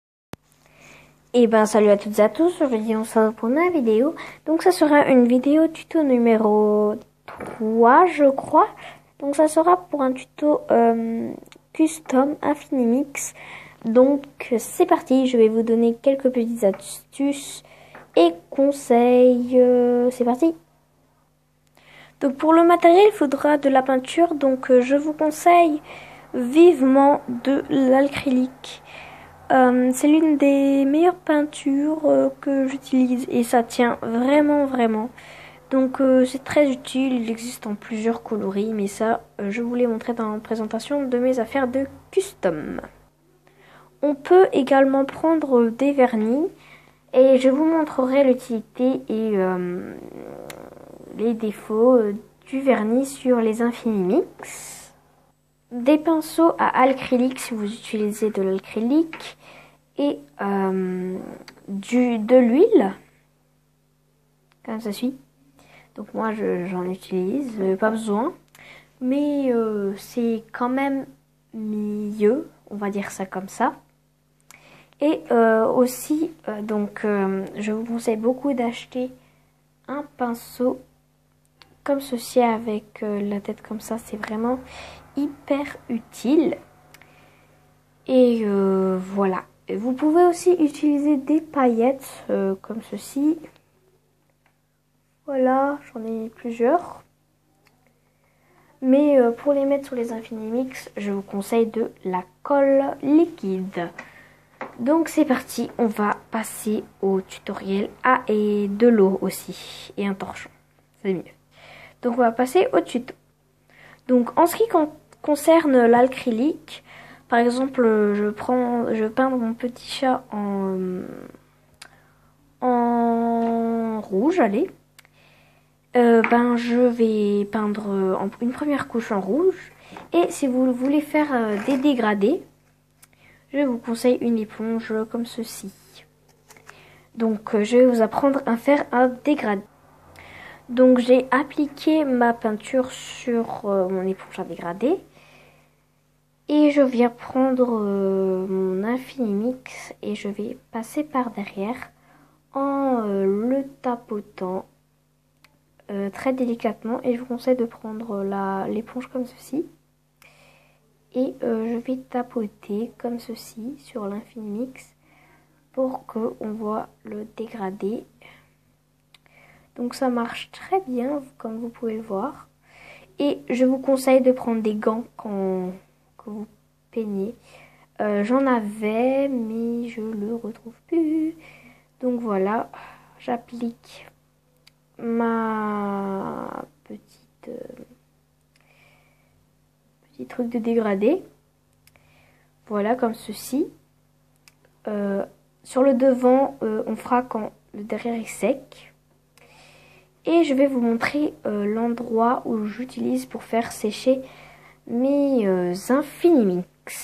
[Tuto n°3] partie 1 ¤custom infinimix¤
Voila tout le monde cette video je l'attendais de la tourner avec impatience ! ;)
Première partie qui est un peu baser sur le degrader , le matériel ect.
Bon visionnage ;)